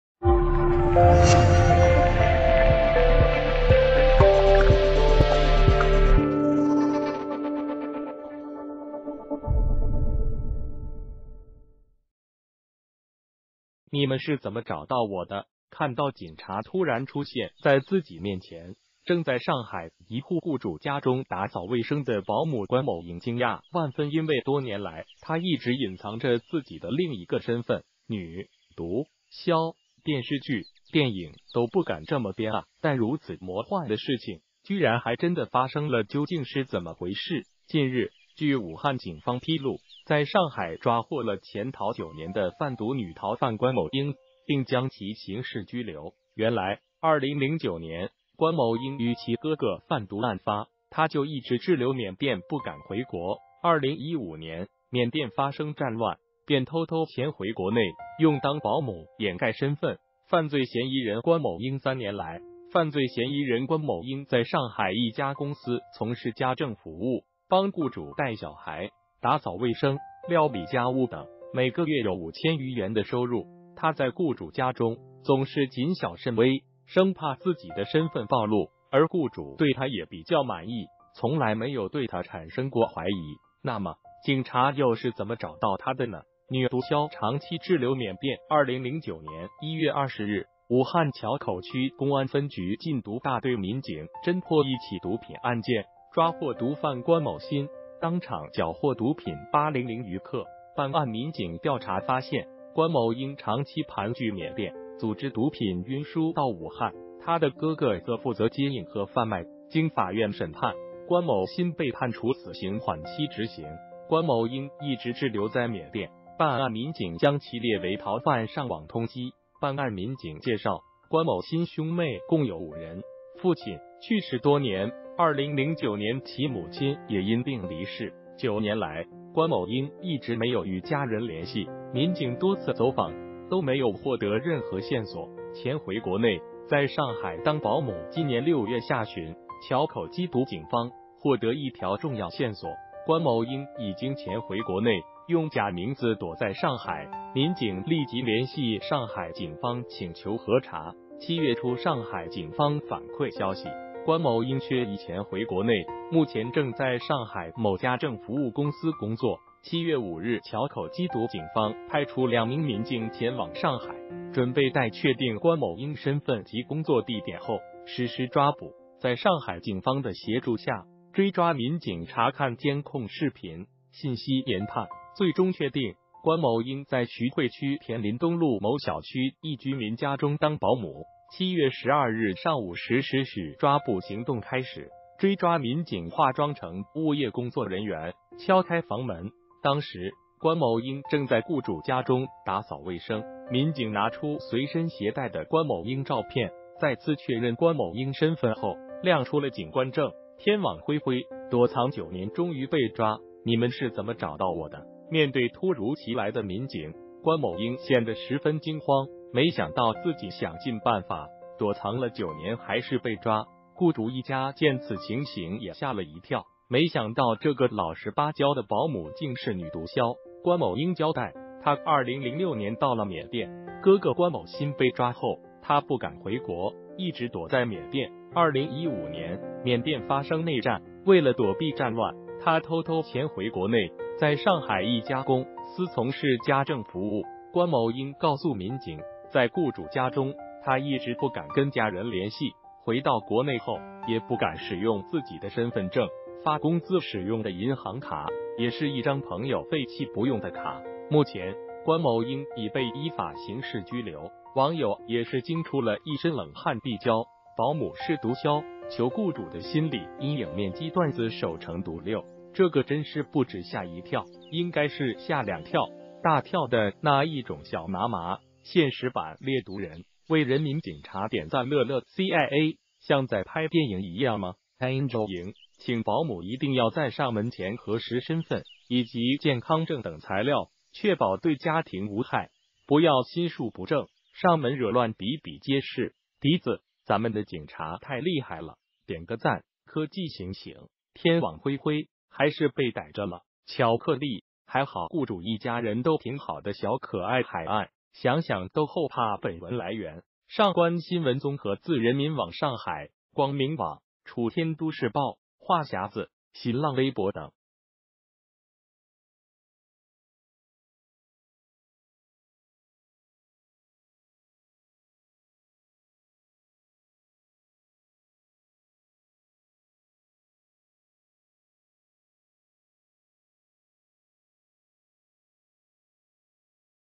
吓出冷汗！沪上一“老实”保姆突然被警察带走，隐藏多年的身份竟是…
谢谢！“你们是怎么找到我的？”看到警察突然出现在自己面前正在上海一户雇主家中打扫卫生的保姆官某英惊讶万分因为多年来她一直隐藏着自己的另一个身份！女！毒！枭！电视剧、电影都不敢这么编啊但如此魔幻的事情居然还真的发生了究竟是怎么回事？近日，据武汉警方披露，在上海抓获了潜逃9年的贩毒女逃犯官某英并将其刑事拘留！原来，2009年官某英与其哥哥贩毒案发，她就一直滞留缅甸不敢回国，2015年，缅甸发生战乱，便偷偷潜回国内，用当保姆掩盖身份……犯罪嫌疑人官某英三年来，犯罪嫌疑人官某英在上海一家公司从事家政服务，帮雇主带小孩、打扫卫生、料理家务等，每个月有5000余元的收入。她在雇主家中，总是谨小慎微，生怕自己的身份暴露。而雇主对她也比较满意，从来没有对她产生过怀疑。那么，警察又是怎么找到她的呢？女毒枭长期滞留缅甸2009年1月20日，武汉硚口区公安分局禁毒大队民警侦破一起毒品案件，抓获毒贩官某新，当场缴获毒品800余克。办案民警调查发现，官某英长期盘踞缅甸，组织毒品运输到武汉，她的哥哥则负责接应和贩卖。经法院审判，官某新被判处死刑缓期执行。官某英一直滞留在缅甸，办案民警将其列为逃犯上网通缉。办案民警介绍，官某新兄妹共有5人，父亲去世多年，2009年其母亲也因病离世。9年来，官某英一直没有与家人联系，民警多次走访都没有获得任何线索。潜回国内，在上海当保姆今年6月下旬，硚口缉毒警方获得一条重要线索：官某英已经潜回国内，用假名字躲在上海。民警立即联系上海警方请求核查。7月初上海警方反馈消息，官某英确已潜回国内，目前正在上海某家政服务公司工作。7月5日硚口缉毒警方派出2名民警前往上海，准备待确定官某英身份及工作地点后，实施抓捕。在上海警方的协助下，追抓民警查看监控视频、信息研判，最终确定：官某英在徐汇区田林东路某小区一居民家中当保姆。7月12日上午10时许抓捕行动开始。追抓民警化妆成物业工作人员，敲开房门。当时，官某英正在雇主家中打扫卫生。民警拿出随身携带的官某英照片，再次确认官某英身份后，亮出了警官证。天网恢恢！躲藏9年终于被抓“你们是怎么找到我的？”面对突如其来的民警，官某英显得十分惊慌，没想到自己想尽办法躲藏了9年还是被抓。雇主一家见此情形也吓了一跳，没想到这个“老实巴交”的保姆竟是女毒枭。官某英交代，她2006年到了缅甸。哥哥官某新被抓后，她不敢回国，一直躲在缅甸。2015年，缅甸发生内战，为了躲避战乱，她偷偷潜回国内，在上海一家公司从事家政服务。官某英告诉民警，在雇主家中，她一直不敢跟家人联系，回到国内后，也不敢使用自己的身份证，发工资使用的银行卡也是一张朋友废弃不用的卡。目前，官某英已被依法刑事拘留。网友也是惊出了一身冷汗避交：保姆是毒枭…求雇主的心理阴影面积段子手成独六：这个真是不止吓一跳，应该是吓两跳，大跳的那一种小麻麻：现实版《猎毒人》，为人民警察点赞乐乐CIA：像在拍电影一样嘛Angel~荧：请保姆一定要在上门前核实身份，以及健康证等材料。确保对家庭无害，不要心术不正，上门惹乱比比皆是！笛子：咱们的警察太厉害了，点个赞！科技醒醒：天网恢恢，还是被逮着了！巧克力：还好雇主一家人都挺好的小可爱海岸：想想都后怕本文来源：上观新闻综合自人民网上海、光明网、楚天都市报、话匣子、新浪微博等